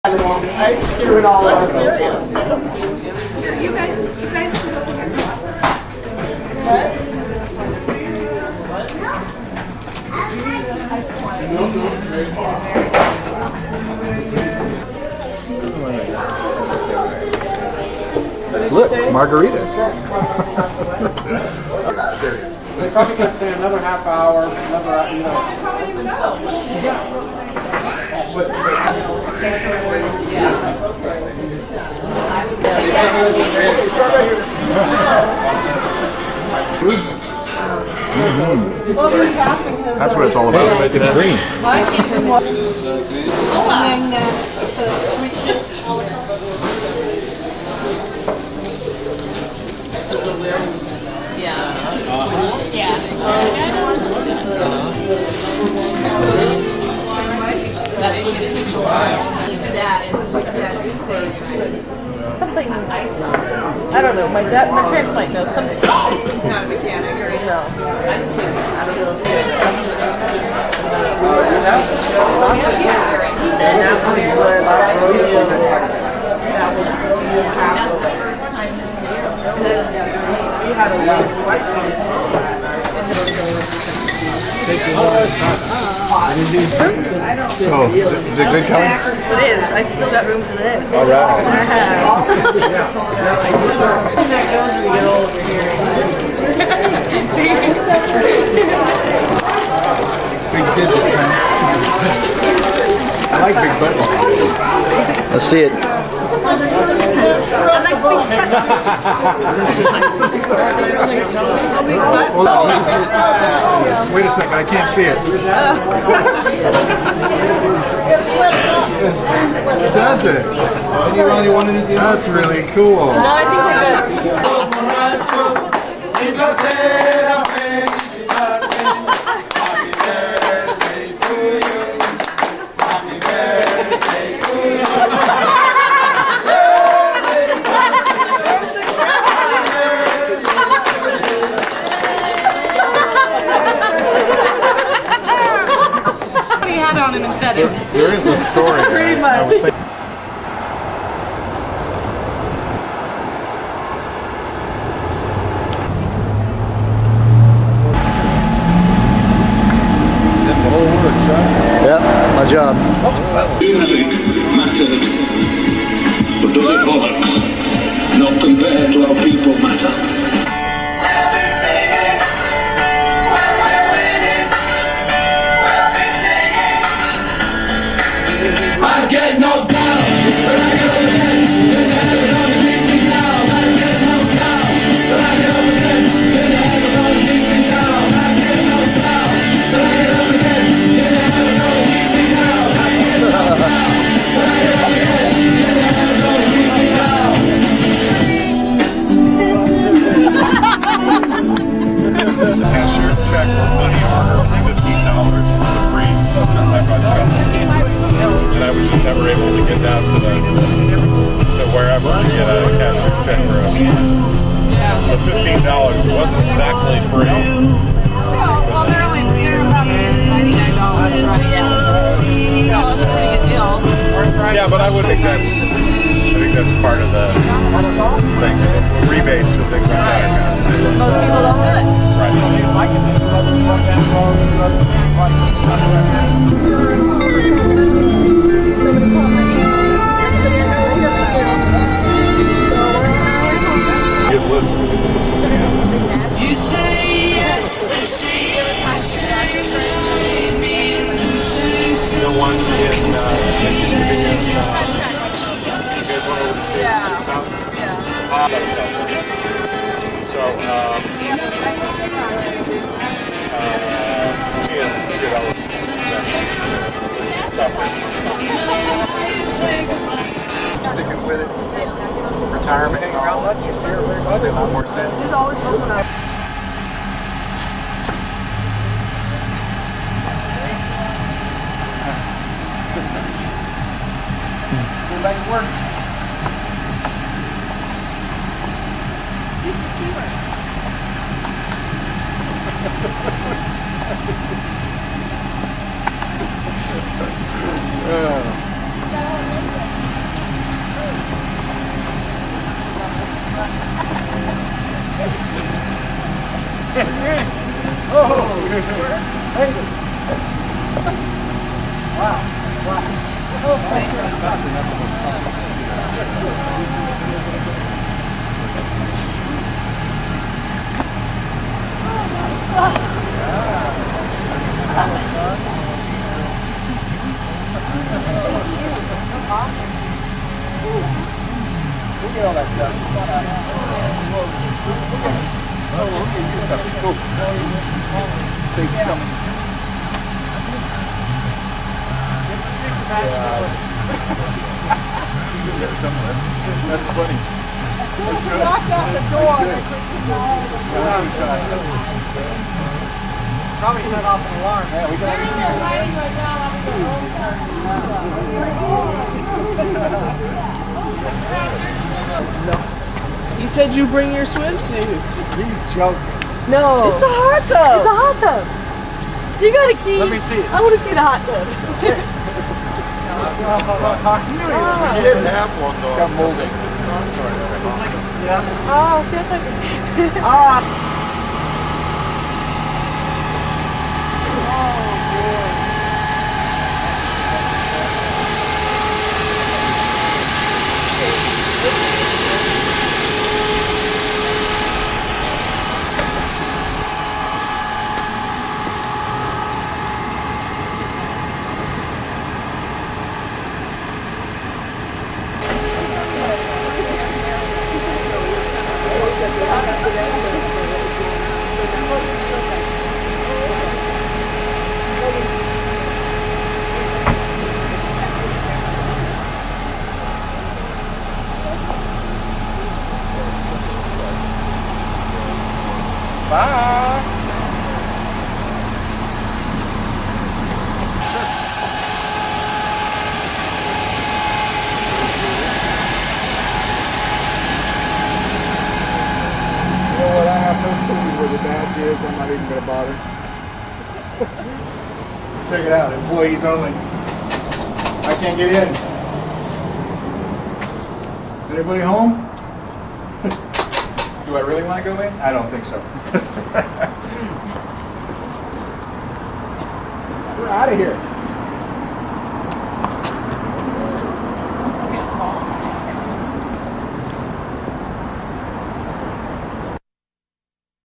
#102 #9. Kim's retirement at El Torito San Francisco 02/16/2007
Kim's retirement from the City of Fremont with family and friends, ride home in limo